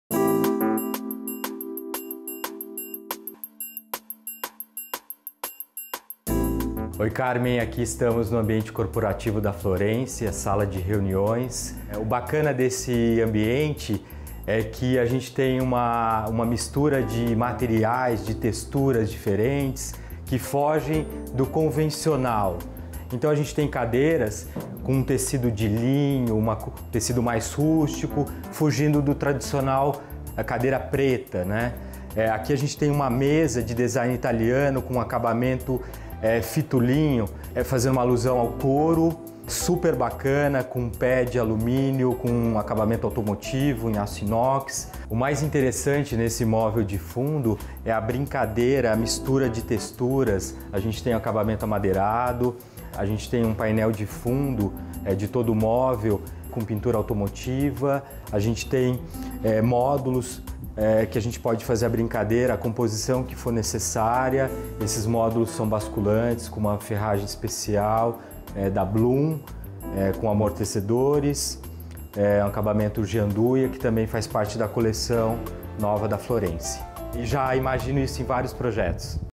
Showroom Florense com Beto Carvalho
O arquiteto Beto Carvalho apresenta mais um ambiente do novo showroom da Florense, em São José dos Campos. Acompanhe!